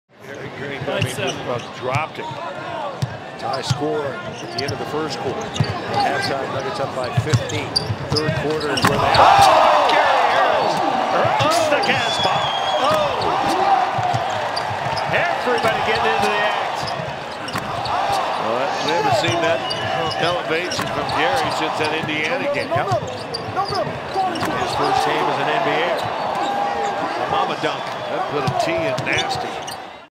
Gary Harris Soars to the Rim for the Poster on Travis Wear
Gary Harris drives to the rack for the athletic smash.